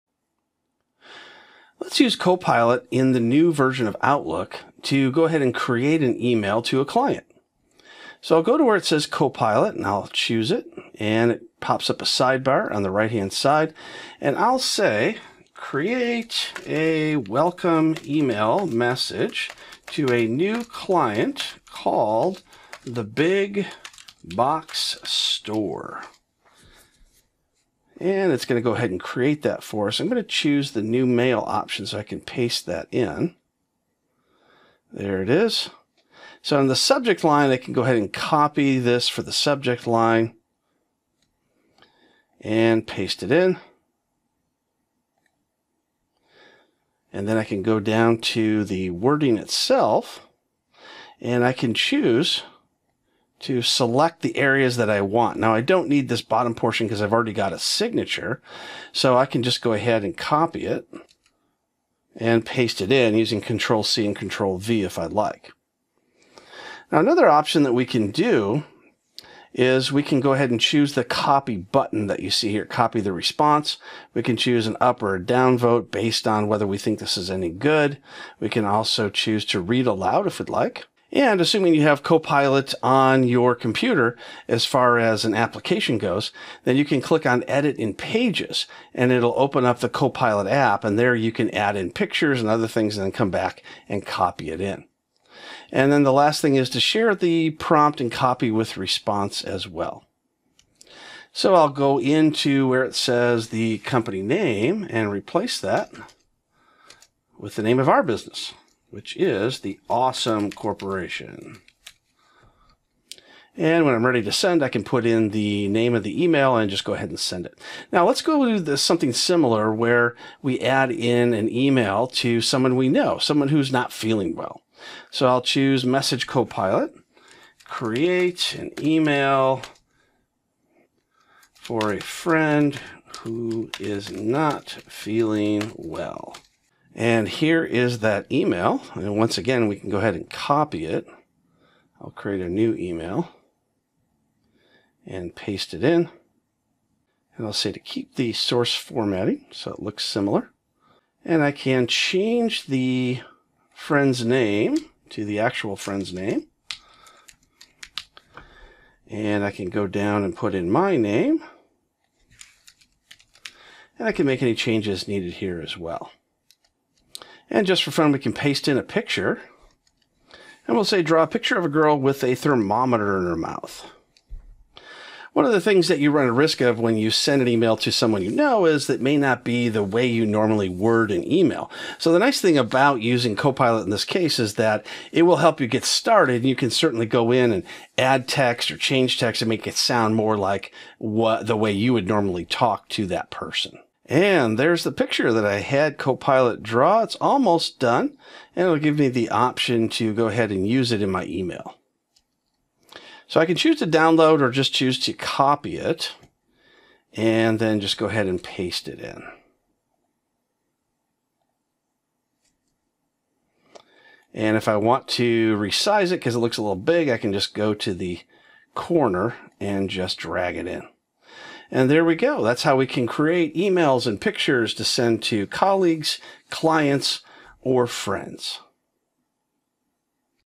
How to use Copilot in New Outlook to draft emails
Professor Robert McMillen shows you how to use Copilot in New Outlook to draft emails.